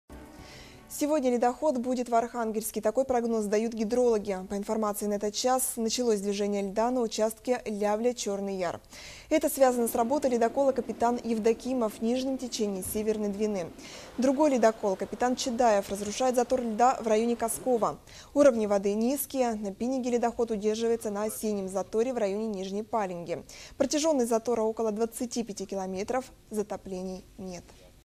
ледокол Капитан Евдокимов 2014
в нижнем течении Северной Двины
На Северной Двине - движение льда на участке Лявля - Чёрный Яр
25 апр. 2014